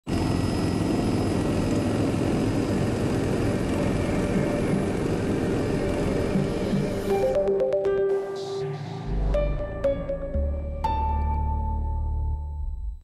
Separador RTP1 2016
Separador da RTP1 emitido em 7 de Abril de 2016. Este grafismo foi utilizado entre 7 de Março de 2016 e 6 de Janeiro de 2017.